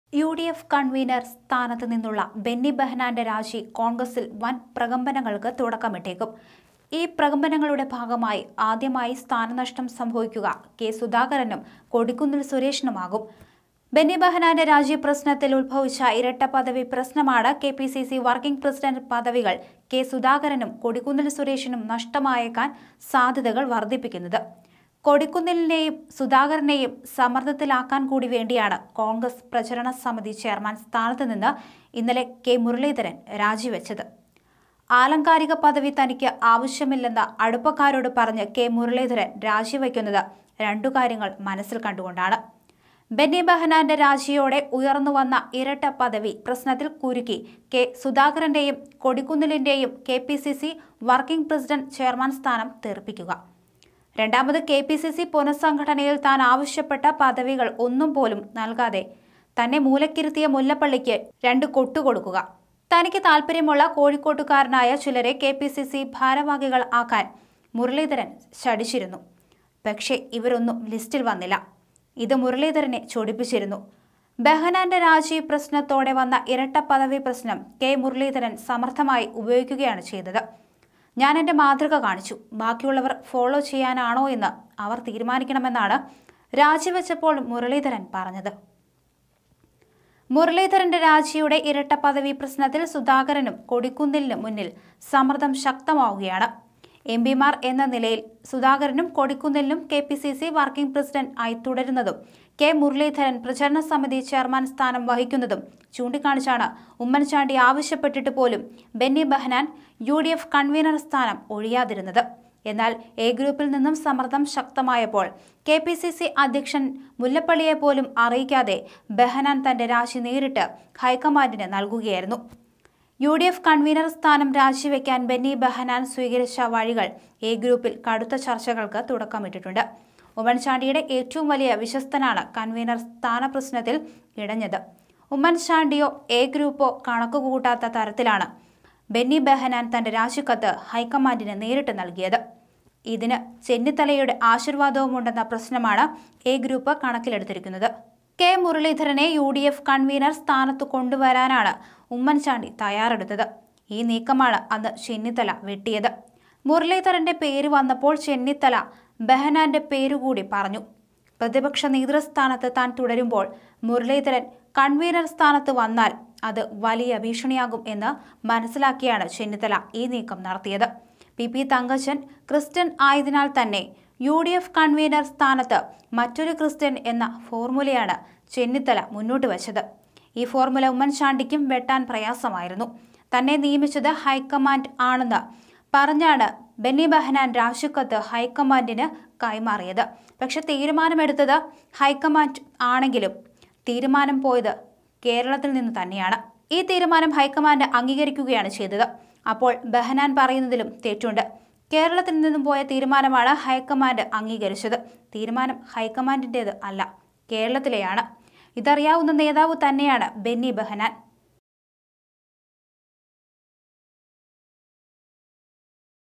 എ-ഐ ഗ്രൂപ്പുകള്‍ പൊരിഞ്ഞ യുദ്ധത്തില്‍; കോണ്‍ഗ്രസില്‍ വീണ്ടും കലഹകാലം l Conflict in Congress again
'ഞാൻ എന്റെ മാതൃക കാണിച്ചു..ബാക്കിയുള്ളവർ ഫോളോ ചെയ്യണോയെന്ന് അവർ തീരുമാനിക്കട്ടെ': ബെന്നി ബഹന്നാൻ യുഡിഎഫ് കൺവീനർ പദവി ഒഴിഞ്ഞതിന് പിന്നാലെ പ്രചാരണ സമിതി ചെയർമാൻ സ്ഥാനം കെ.മുരളീധരൻ രാജി വച്ചത് കോൺഗ്രസിലെ ഇരട്ടപ്പദവി പ്രശ്‌നം ചൂടാക്കാൻ; കെ.സുധാകരനെയും കൊടിക്കുന്നിലിനെയും ലക്ഷ്യമിട്ടുള്ള നീക്കത്തിൽ ഒരമ്പ് മുല്ലപ്പള്ളിക്ക് നേരേയും; അടുത്ത മുഖ്യമന്ത്രി ആരാകണമെന്ന മത്സരവും മുറുകിയതോടെ എ-ഐ ഗ്രൂപ്പുകൾ പൊരിഞ്ഞ യുദ്ധത്തിൽ; കോൺഗ്രസിൽ വീണ്ടും കലഹകാലം